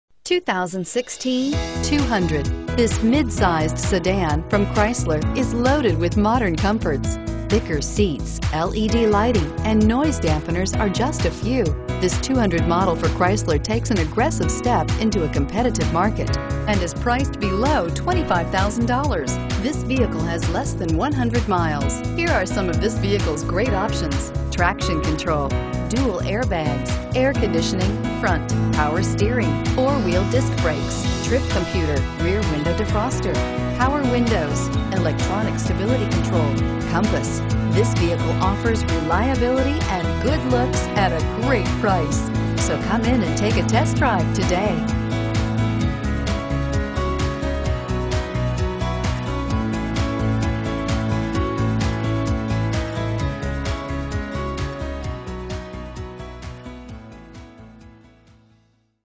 2016 Chrysler 200 Richardson TX GN137467
2016 Chrysler 200 Limited

Richardson CDJR
1321 N Central Expressway

You'll love this 2016 Chrysler 200. This is a sedan you'll want to take home. With 1 miles, it features Automatic transmission and an exterior color of Granite Crystal Metallic Clearcoat.